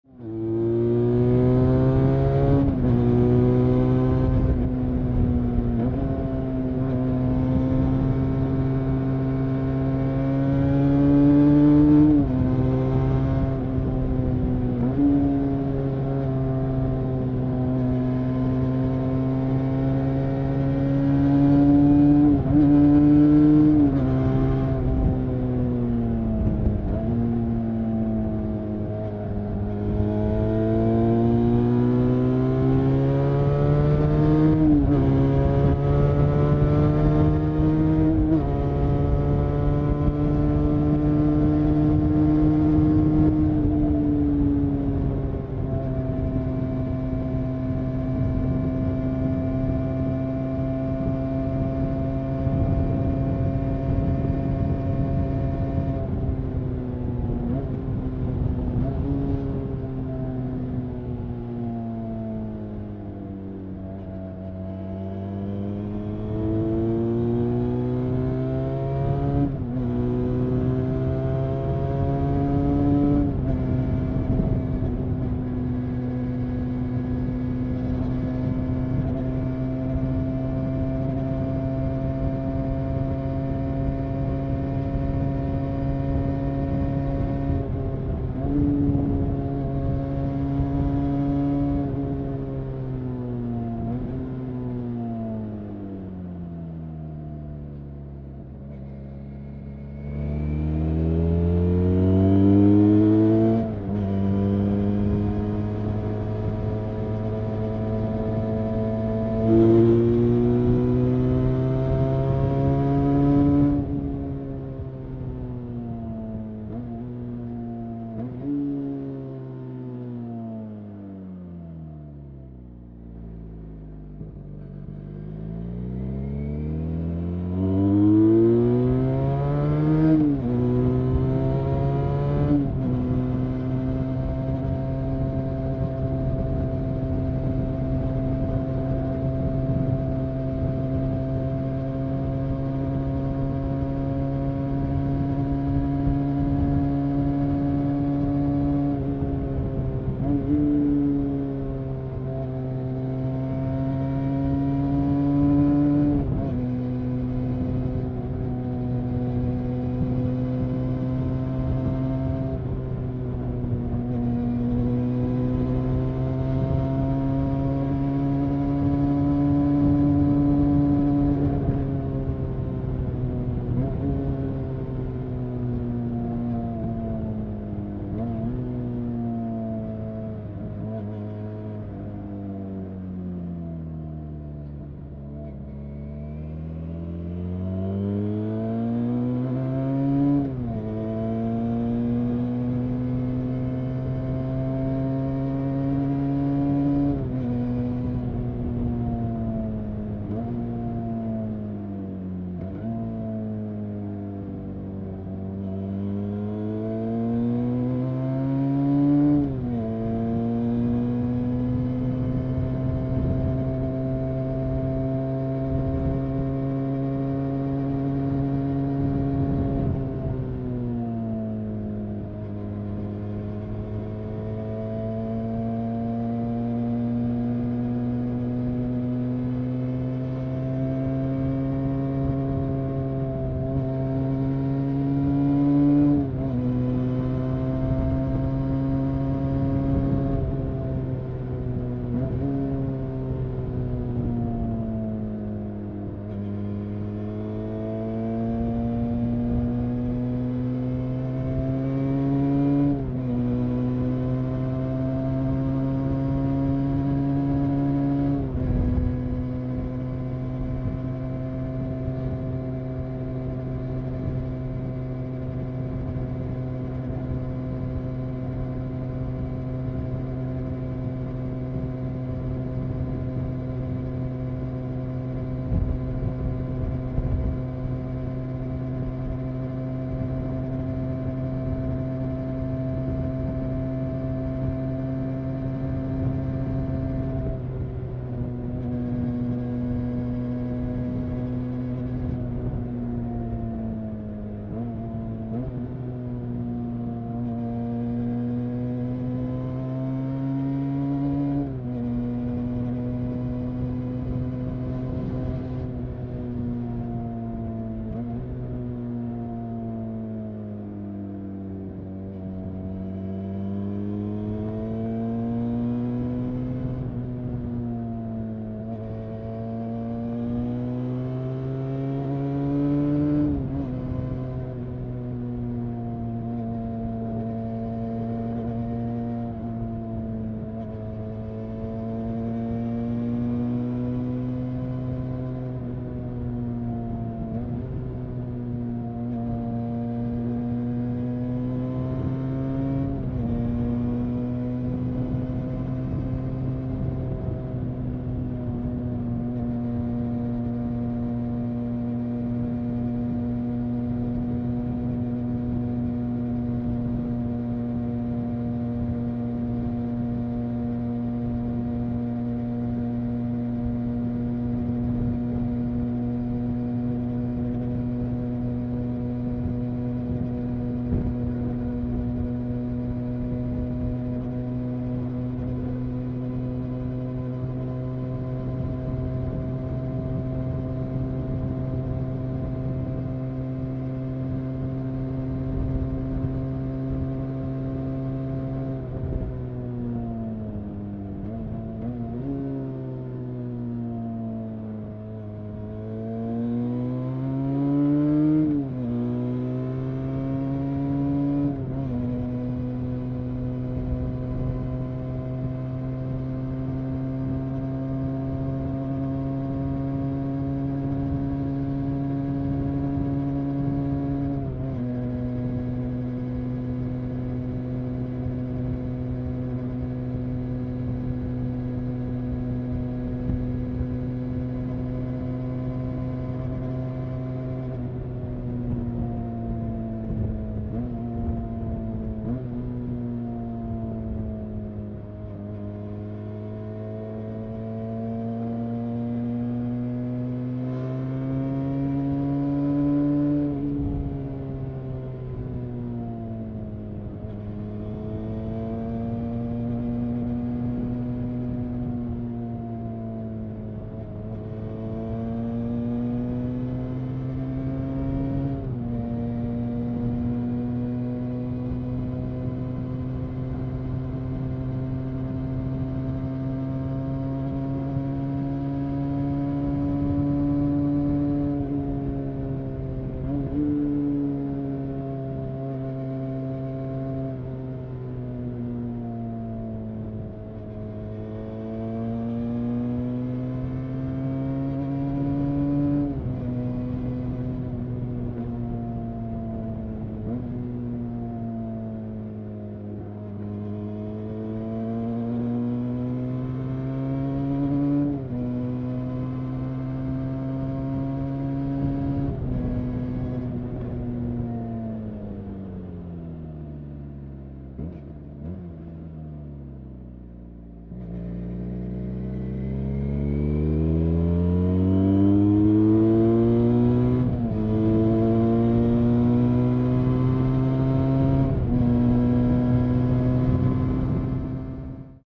Benelli 302s [RAW ONBOARD] stock exhaust
ENG: Hi everyone, one of my favorite roads, I know it's full of holes, dirty and narrow, but for some reason I like it a lot, especially on small bikes. Watch the movie, enjoy.

PL:Cześć wszystkim, jedna z moich ulubionych dróg, wiem ze jest dziurawa, brudna i wąska, ale z jakiegoś względu bardzo ją lubię, zwłaszcza na małych motocyklach. Zapraszam na film, miłego oglądania.